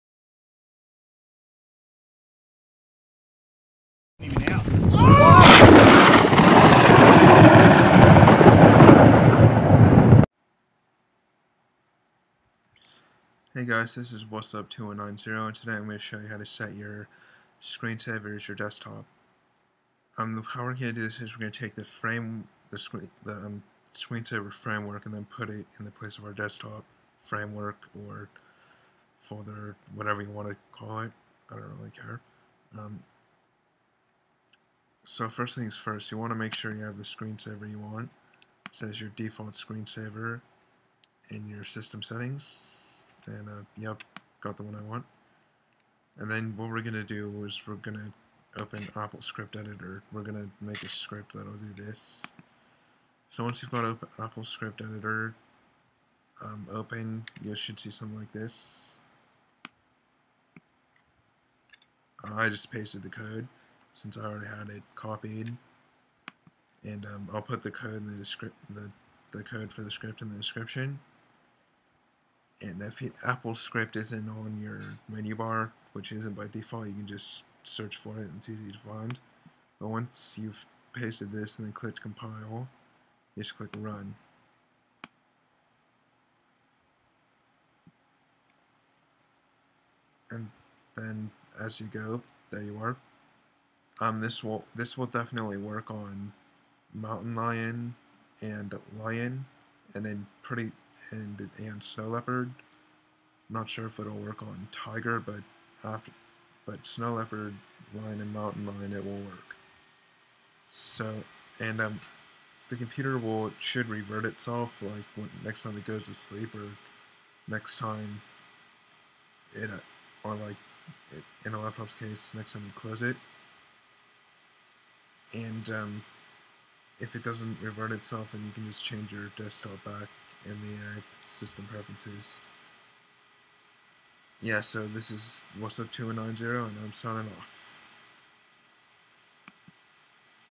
How To Set Your Screensaver As Your Desktop (Mac OSX)
I show you how to set your screensaver as your desktop in mac osx. Here is the code for the Applescript Editor: do shell script "/System/Library/Frameworks/ScreenSaver.framework/Resources/ScreenSaverEngine.app/Contents/MacOS/ScreenSaverEngine -background"
end

If you don't want to have to do this again, save it as a script. That way, in future, all you will have to do is double click the script.